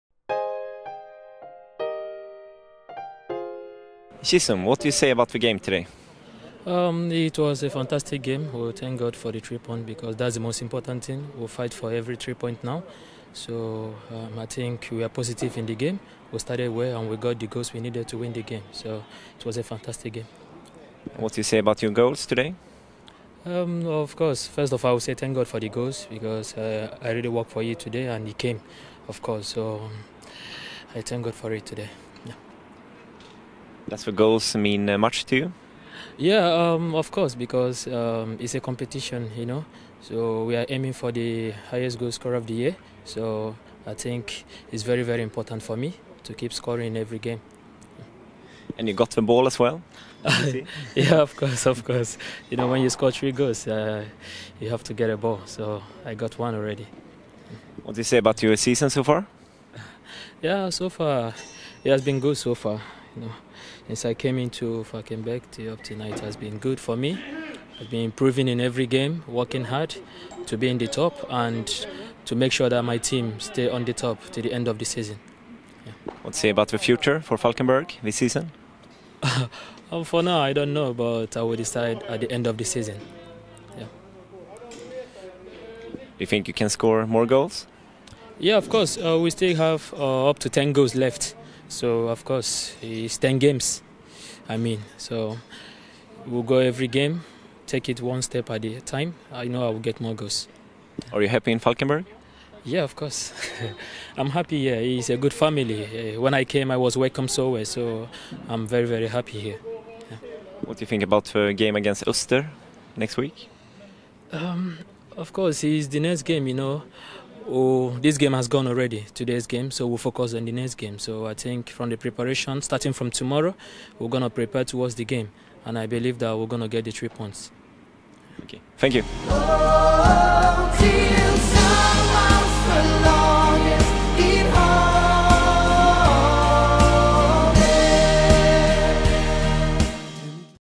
Matchintervju, Chisom Egbuchulam Falkenbergs FF - Degerfors IF, Superettan 2018
Intervju med Chisom Egbuchulam efter matchen mellan Falkenbergs FF - Degerfors IF i Superettan 2018